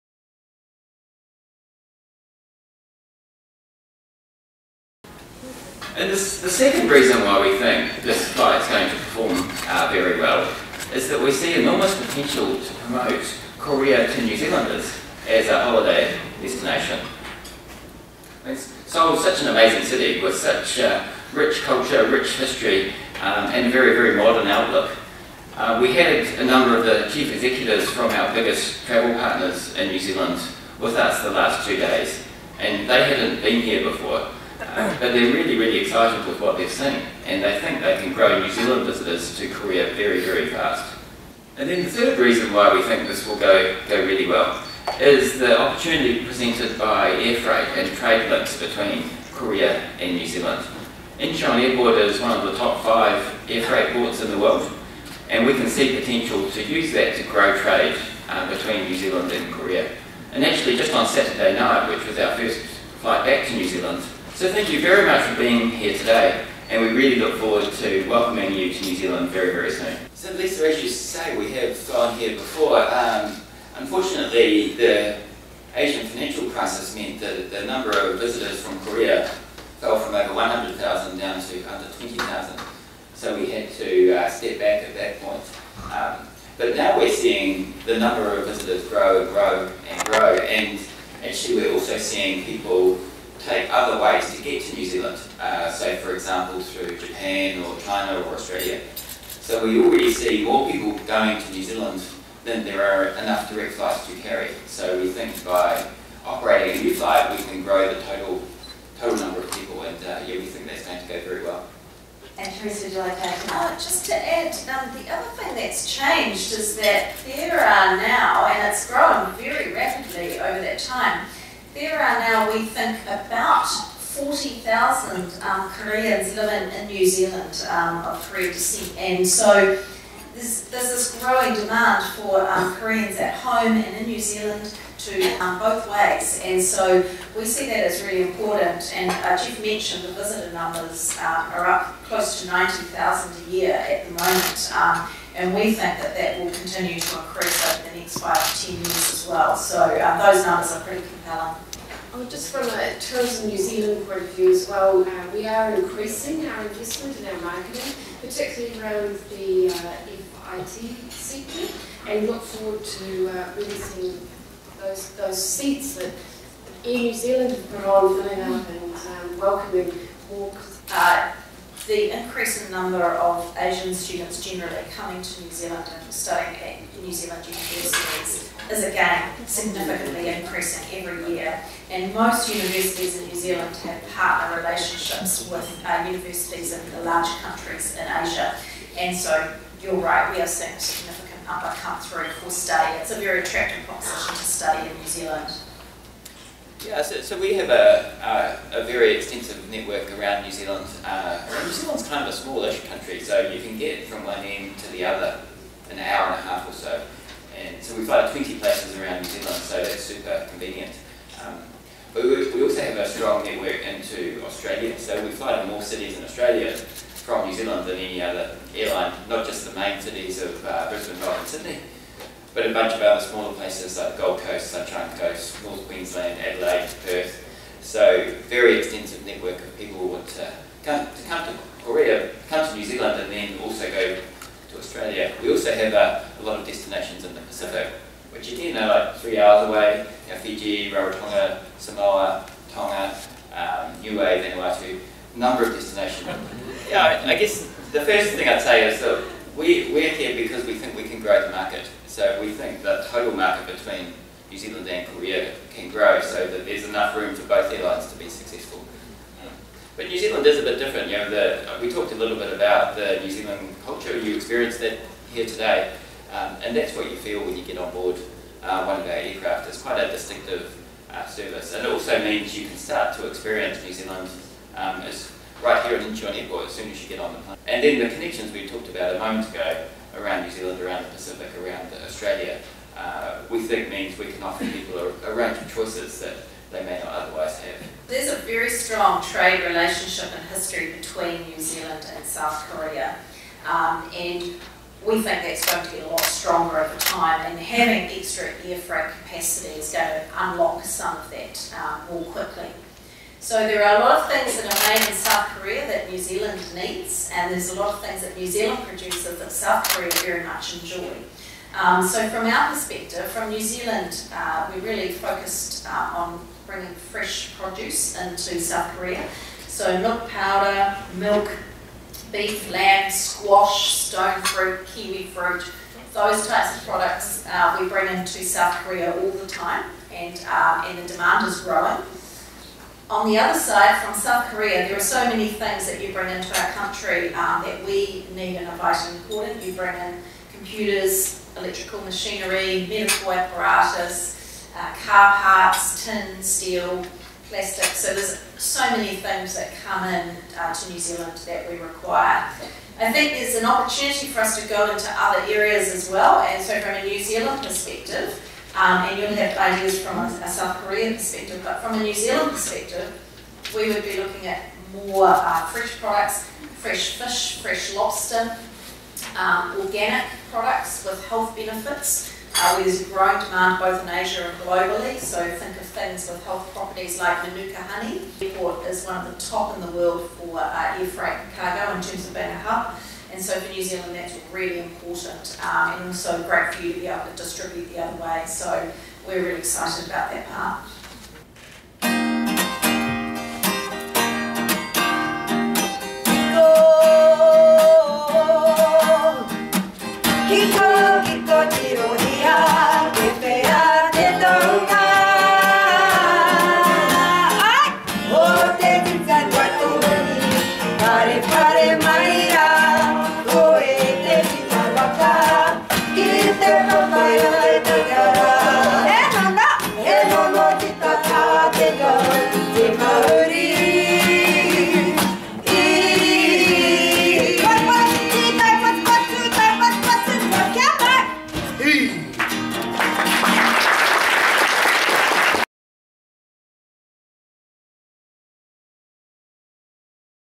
에어뉴질랜드, 인천-오클랜드 직항 신규 취항 / 현장을가다
뉴질랜드 하늘길 22년만에 다시 열려
보잉787-9 드림라이너 투입 "한국 시장 본격 공략"
오클랜드 직항 비롯 20여개 도시 경유편 제공